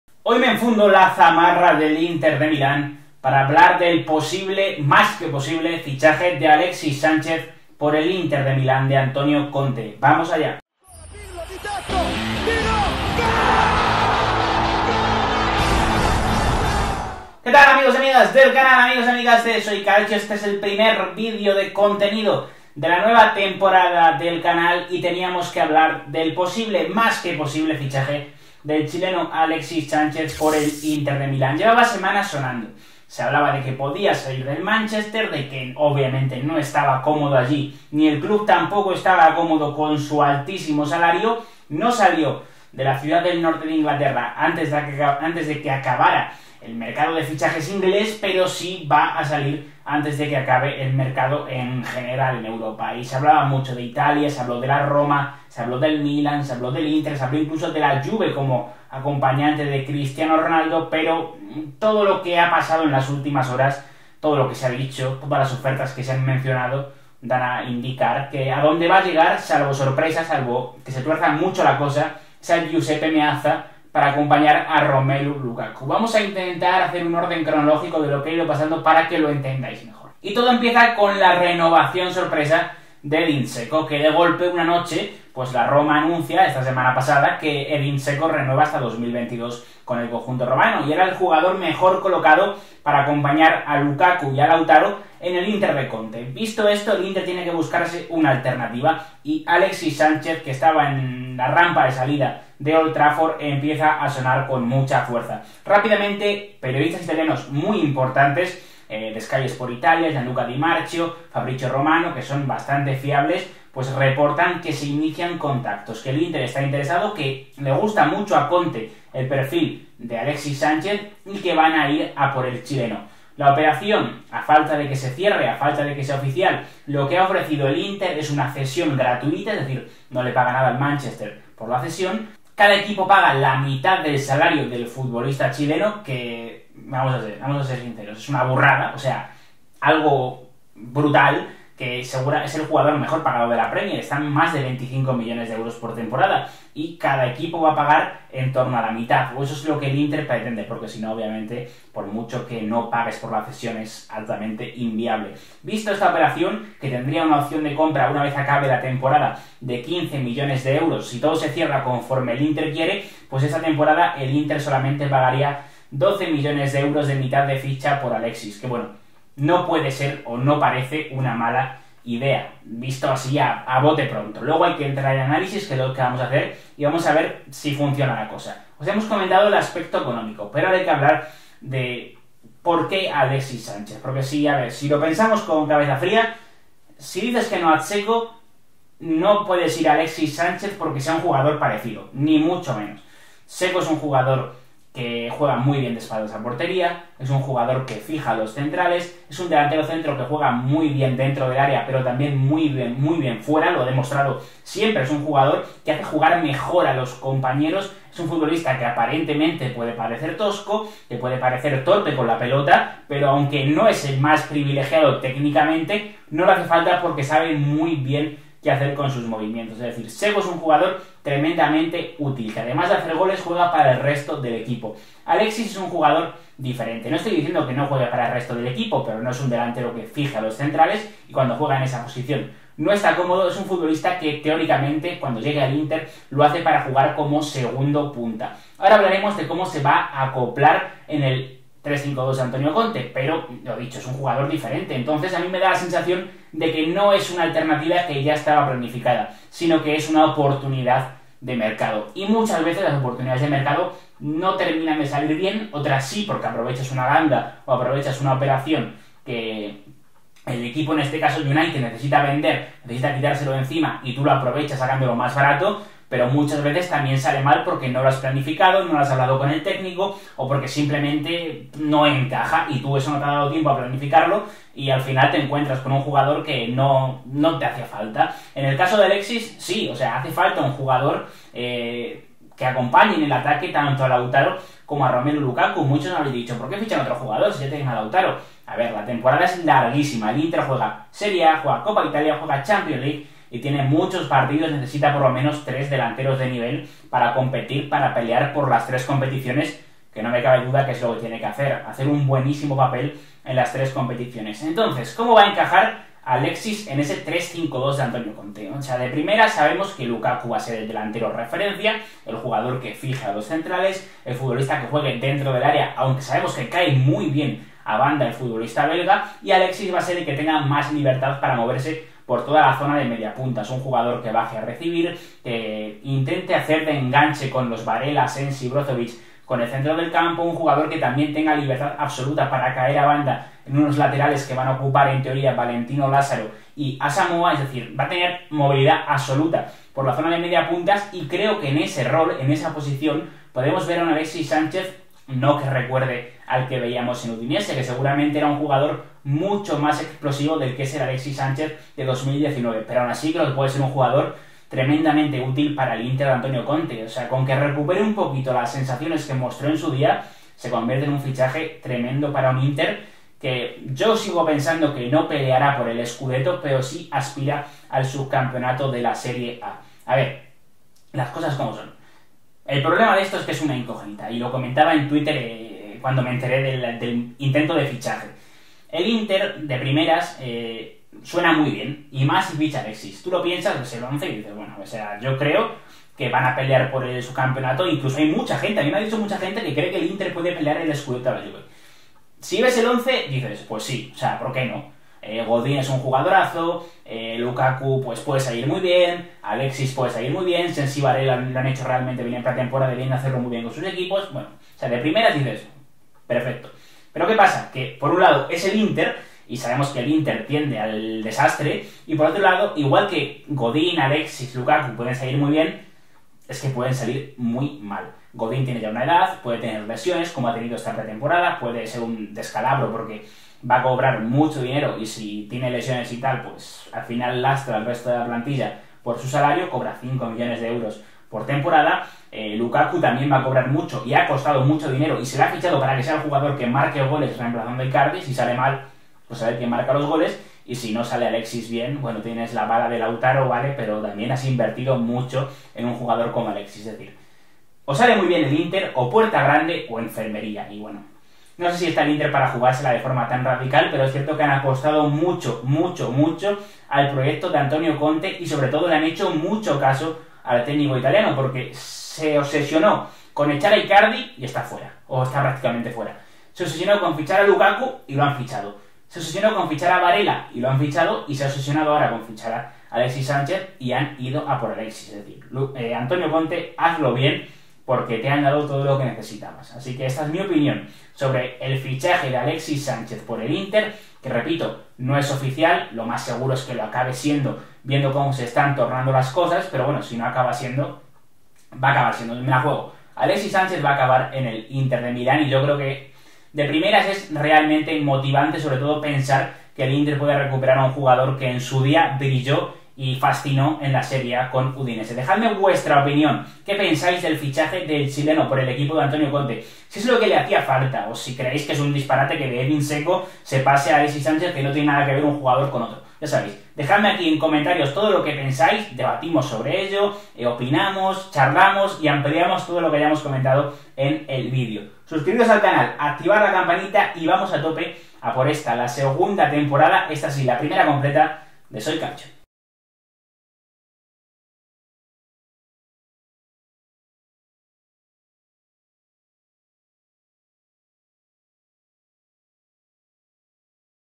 ALEXIS SÁNCHEZ, LA ¿GUINDA? AL INTER DE MILÁN DE CONTE
Alexis Sánchez, a un paso del Inter. El jugador chileno llegaría al Meazza en calidad de cedido, pero... A falta de confirmarse... ¿Cómo encajaría en el esquema de Antonio Conte? Es hora de analizarlo.